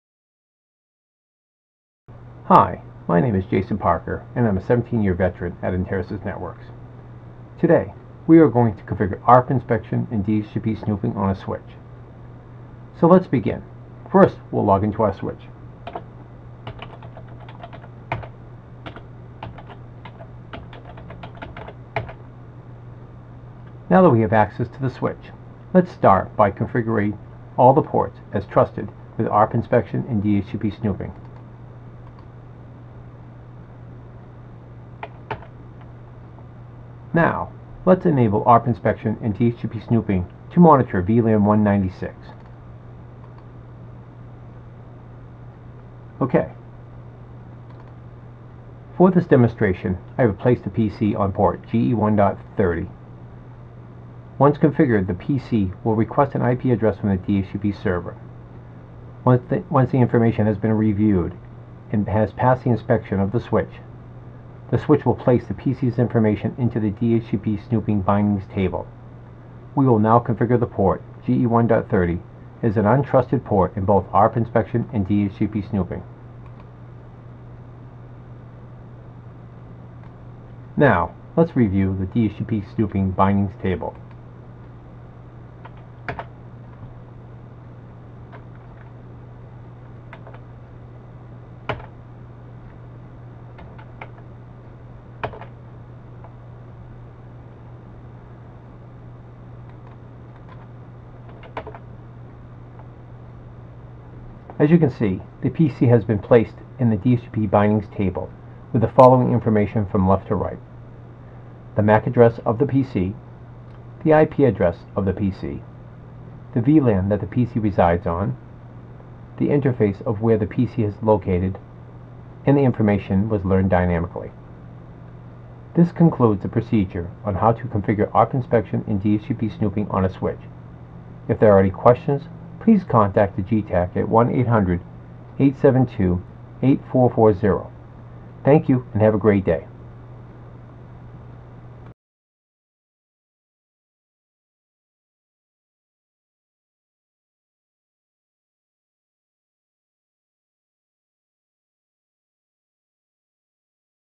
How to configure ARP Inspection and DHCP snooping
In this How To tech tutorial, Enterasys GTAC veteran Jason Parker describes how to configure ARPInspection and DHCPsnooping your network switches.